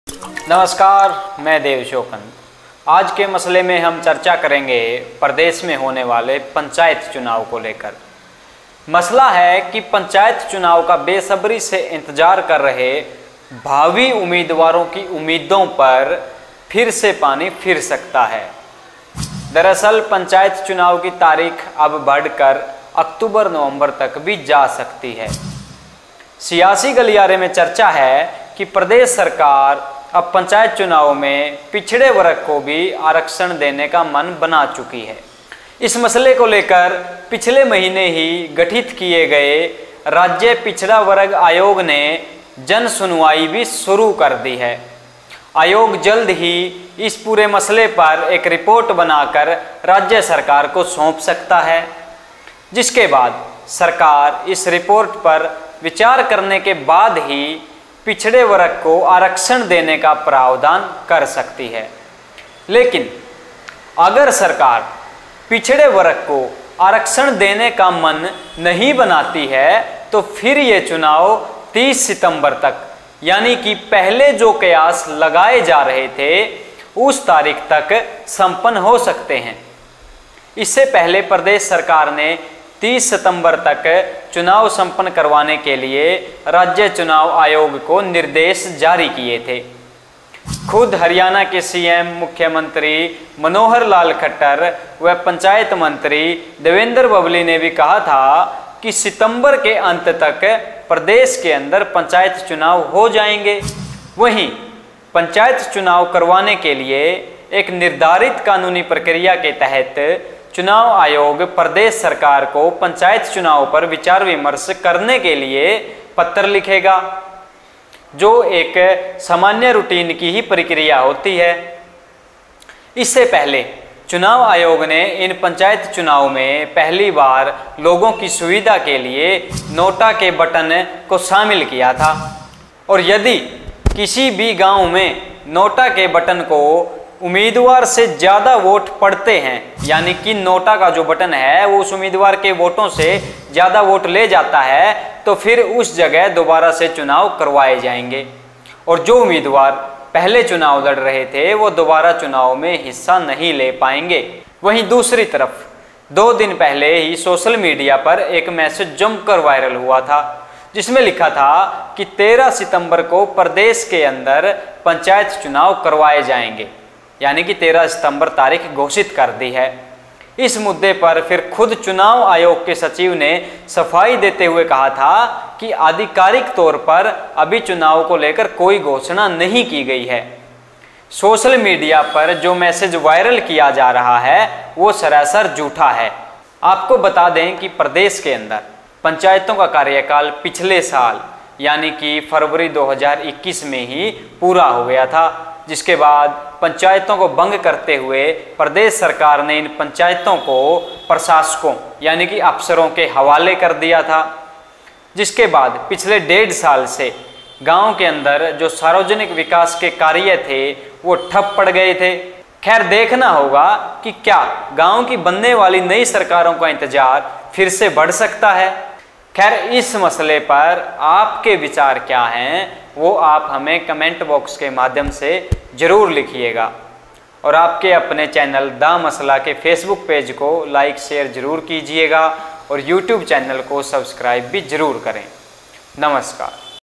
थोड़ा और टल सकते हैं पंचायती चुनाव ! देखिए क्यों ! Haryana Panchayat Elections
Host - Dev Sheokand